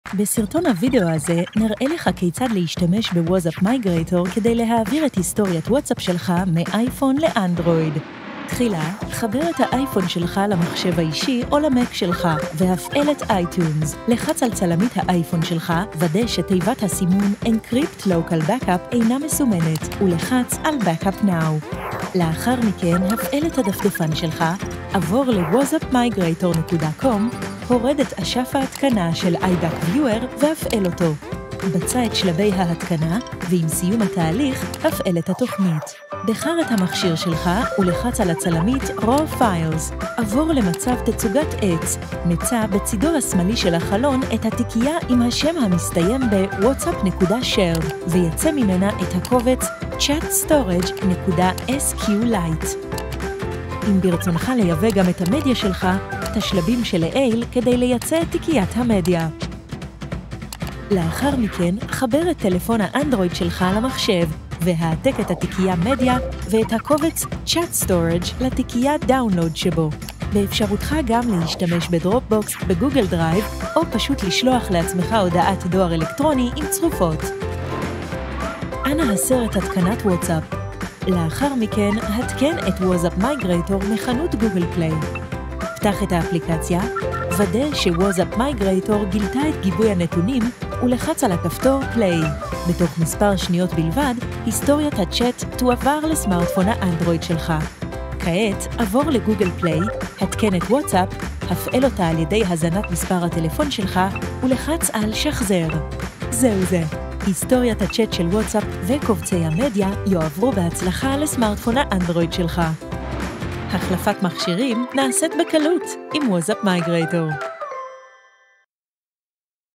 WazzapMigrator video tutorial - Hebrew - OUTDATED
העבירו את הודעות WhatsApp שלכם מ-iPhone ל-Android